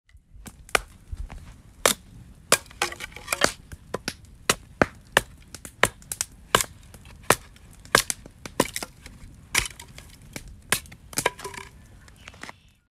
Splitting wood while winter camping: Made with Sound Journal
This is a photo combined with an audio field recording using Sound Journal app for iPhone

Capture your own with Sound Journal app for iPhone.